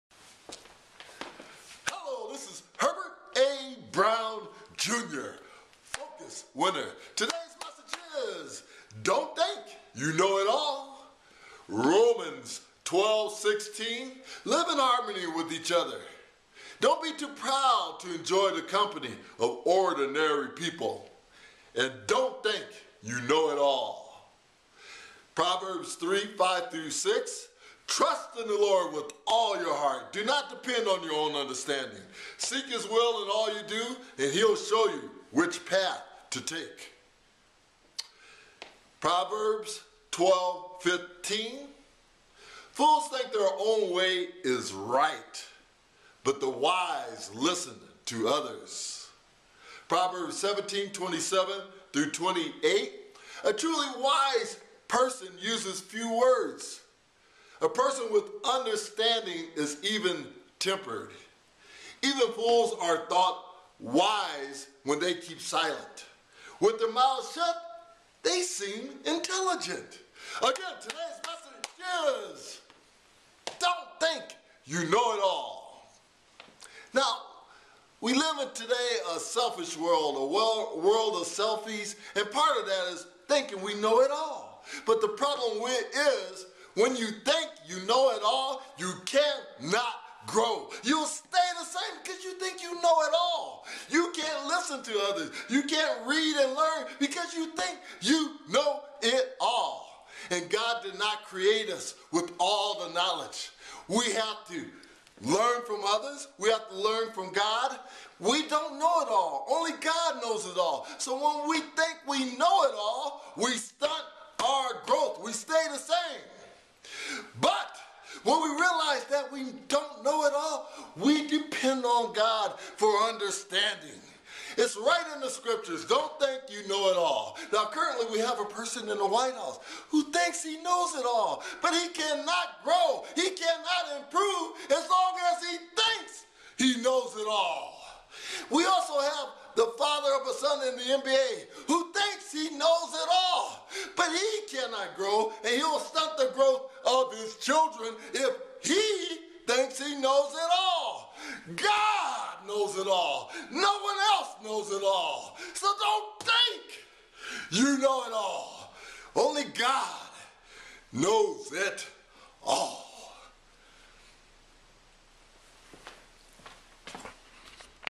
Don't Think You Know It All!!
So many of us think we KNOW IT ALL!  Others of us know someone who thinks they know it all!  God did not create us to know everything or we would not to depend on HIM!  God created us to learn from others, and attain knowledge from HIM!  Once we realize this we do not stunt our own growth by going through life thinking we know it all when in fact we don't!  So stay humble and realize that the only way to grow in life is to learn from others and most importantly attain wisdom from God!!